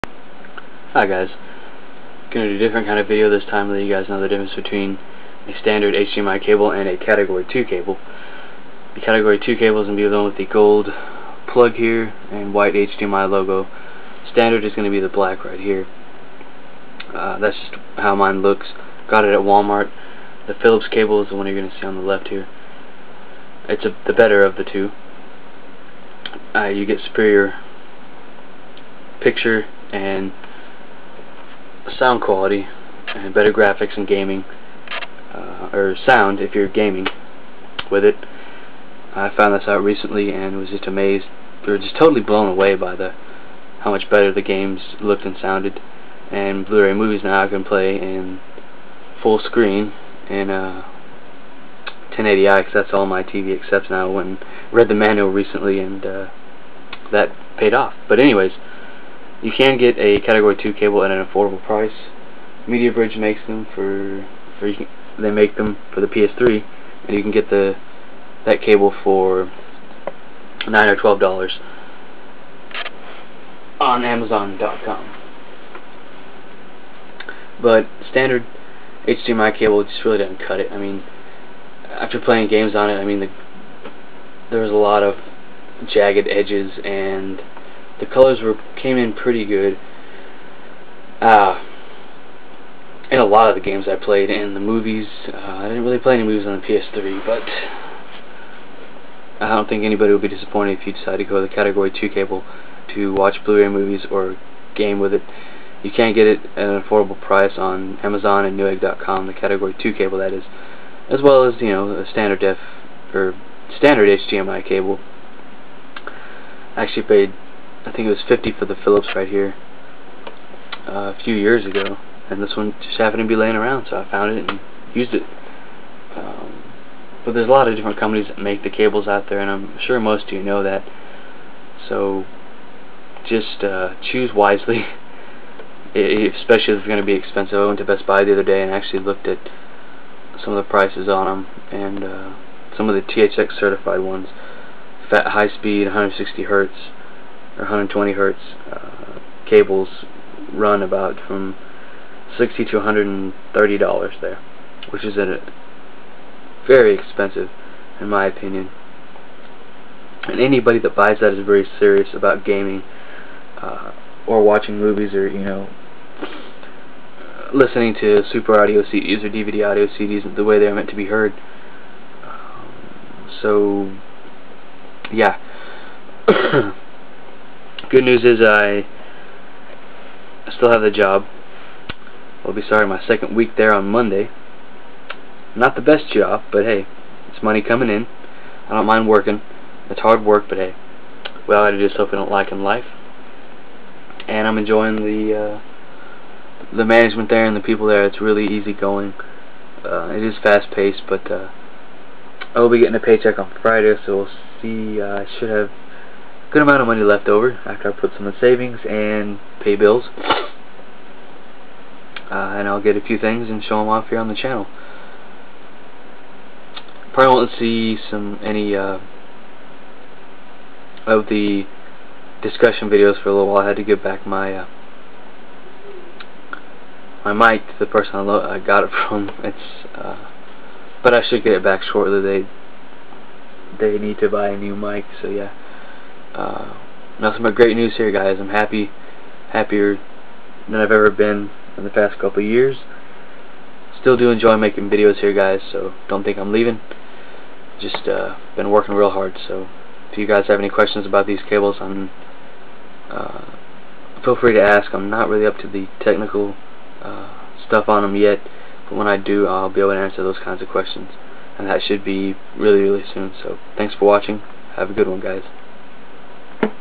Difference Between Standard and Catagory 2 HDMI cable
Check my weekly bulliten posts here on my channel.I keep you guys up to date when I am putting together videos.

 In this video I talk about the difference between your run of the mill HDMI cable and a Catagory 2 cable.A standard or cheap HDMI cable you will get what ya pay for with it. With a Catagory 2 Cable you will get much better picture quality and sound. Feel free to drop a comment or question. Questions will be answered ASAP!